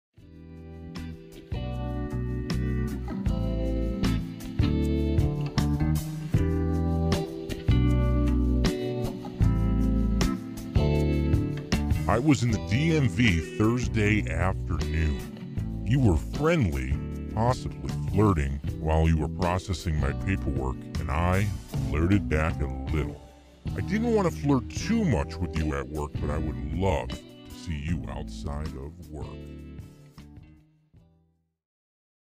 Utica Craigslists Missed Connections- Herkimer DMV
Today's episode takes us out to the Valley to find love at the DMV.